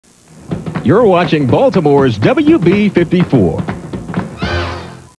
WB 54 WNUV Baltimore Ident (October 10,1999)
Short Ident for WB 54 WNUV Baltimore with the announcer saying “You’re Watching Baltimore’s WB 54”. This ident aired between commercial breaks during the airing of the 3rd Episode of the 2nd of Felicity “Ancient History” which aired on The WB Sunday Night October 10,1999 on then WB Network Affiliate WNUV WB 54 in Baltimore, Maryland. Shoutout to AZERTY’s VHS Archive for the footage since it came from a commercial break on The WB Sunday Night October 10,1999 on WB 54 WNUV Baltimore.